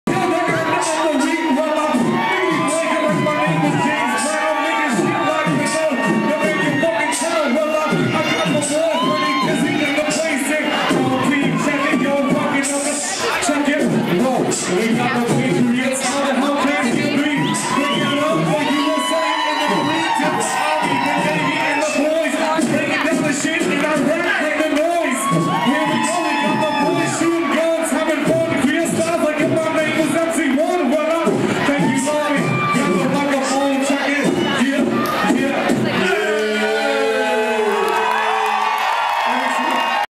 Larryoke 2008 - Lonnie Paxton Beatbox
New England Patriots' Lonnie Paxton beatboxing at Larryoke 2008.